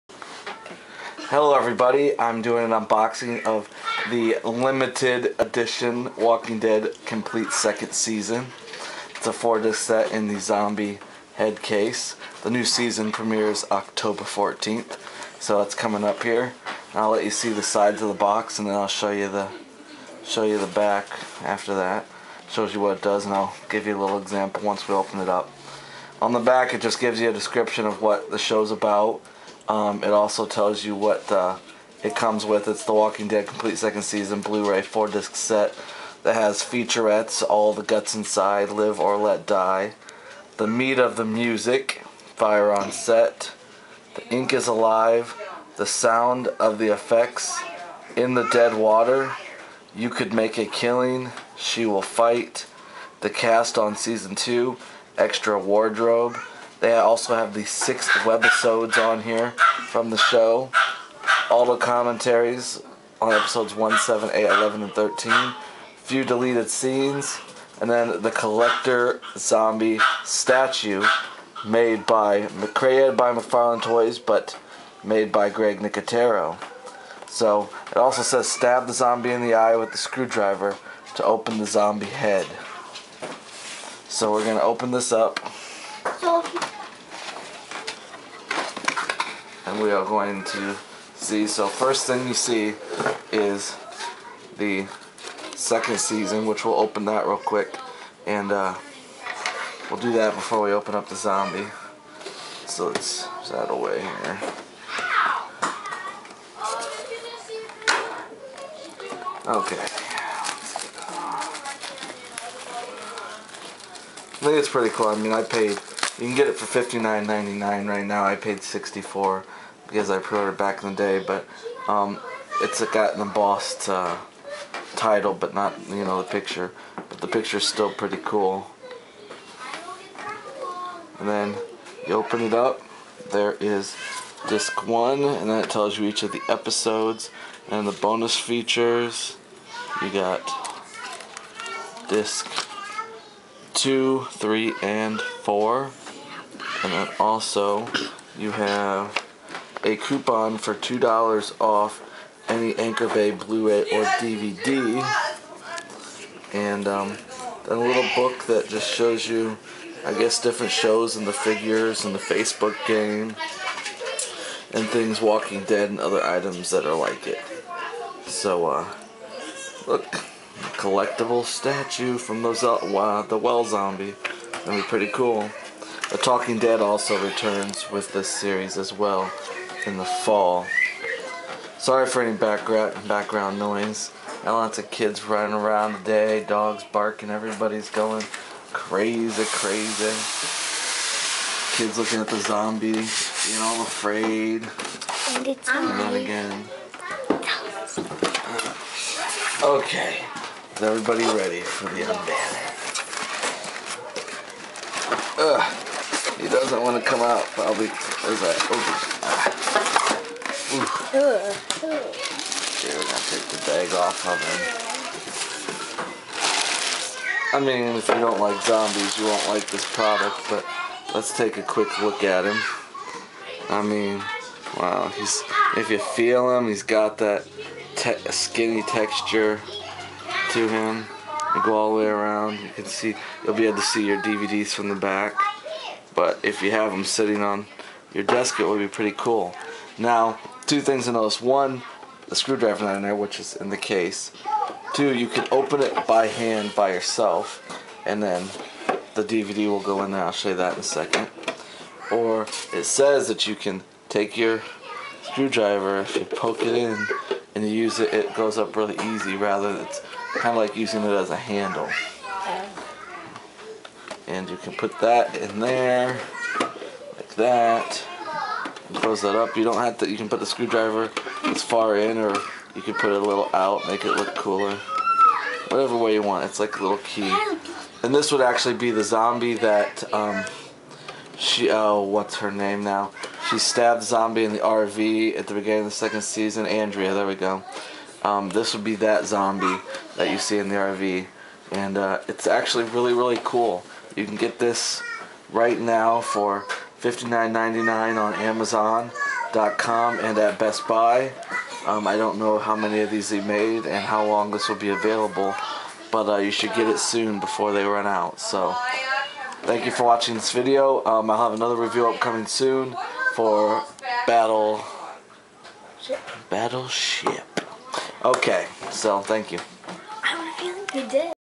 Walking Dead Season 2 Blu-Ray Limited Edtion Unboxing
Today we are unboxing the season 2 limited edition of walking dead, this is the one with the zombie head case. its very neat. you can get this for 59.99 or 64.99 depending on the retailer you get it. i love zombies, i love the walking dead and this dvd set.